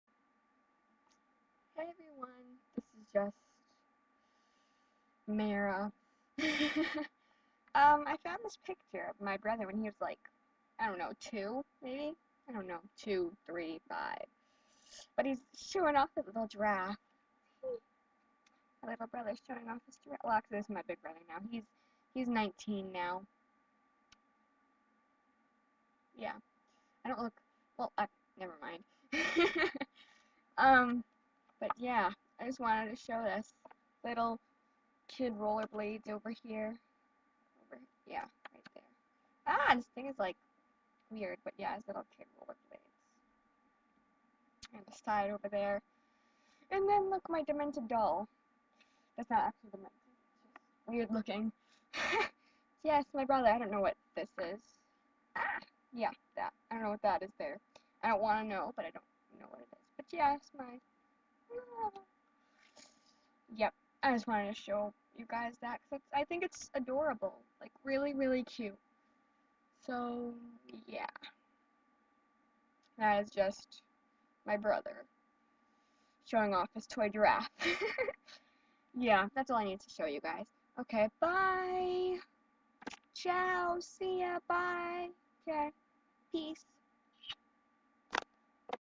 My Brother Showing Off His Giraffe!?
This Is A Picture Of My Brother when he was like... 2? He Was Showing Off His Toy Giraffe... And Sorry, The Mic Isnt So Great.